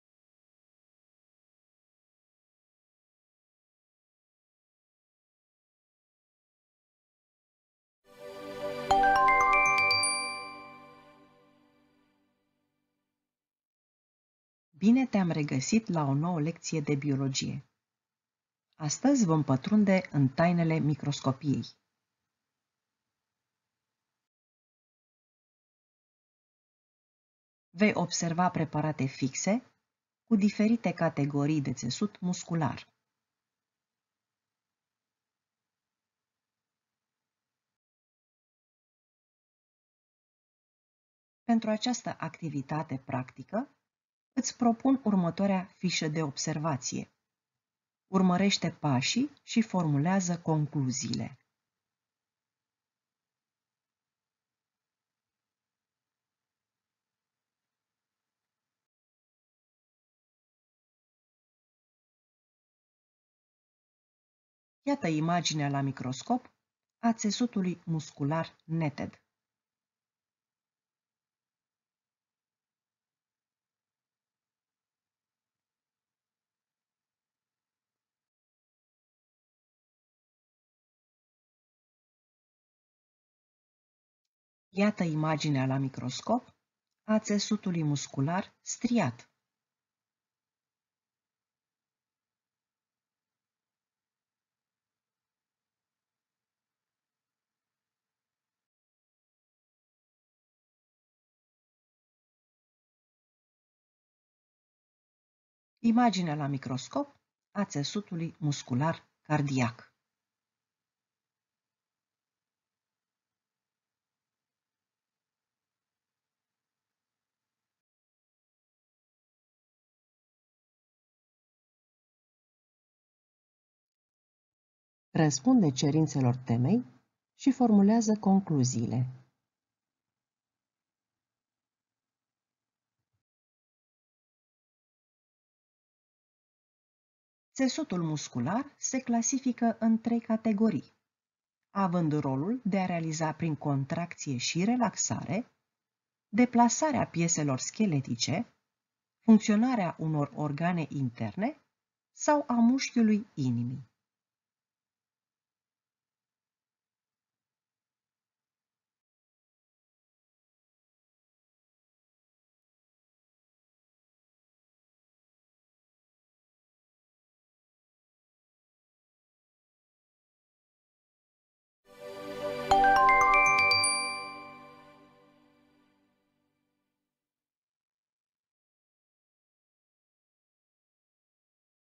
Observații microscopice asupra țesuturilor animale pe preparate fixe țesut muscular
Prezentarea componentelor microscopului optic.
Prezentarea modului de funcționare a unui microscop optic.

Autor: Marinescu Mirela